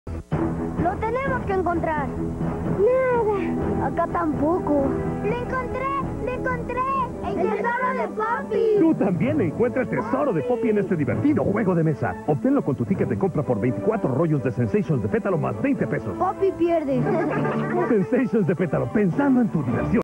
Comerciales mexicanos: Petalo Sensations 2003 2
El tesoro de poppy con un juego de mesa para conseguirlo se tenia que tener 20 rollos más $20